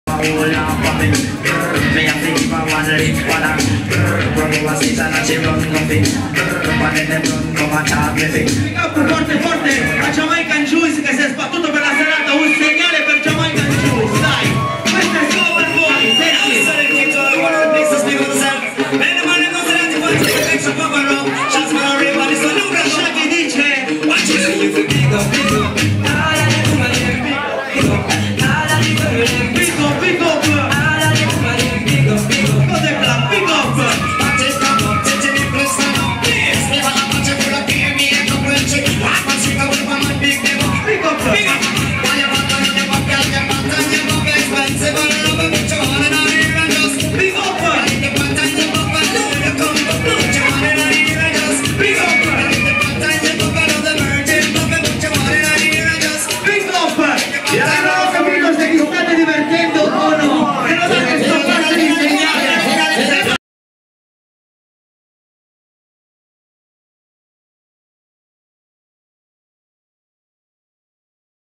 BIGUP - Reggae e DanceHall_30 Settembre
INAGURAZIONE BIG UP

Roots Ragga Dancehall

Line up:

POWA FLOWA FAMILY

PUPPIES SOUND

RISING HOPE